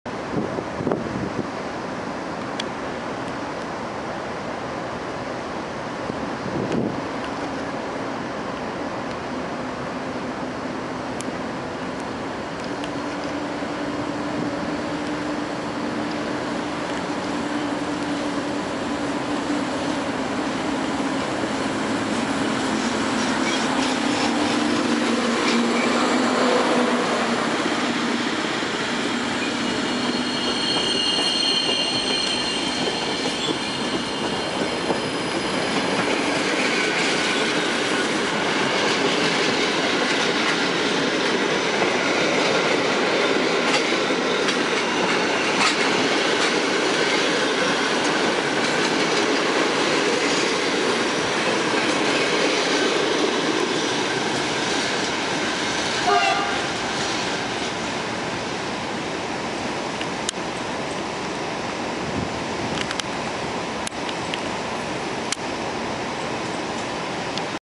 EIC "Kossak" z Krakowa Głównego wyrusza na tory postojowe Warszawa Grochów
Pociąg EIC "Kossak" z Krakowa Głównego który skończył bieg na stacji docelowej Warszawa Wschodnia wyrusza na tory postojowe Warszawa Grochów skład tego pociągu później odjedzie jako EIC "Pieniny" do Przemyśla Głównego i Nowego Sącza

Data nagrywania filmu: 15.08.10r
Autor nagrywania filmu: mkfilmykolejowe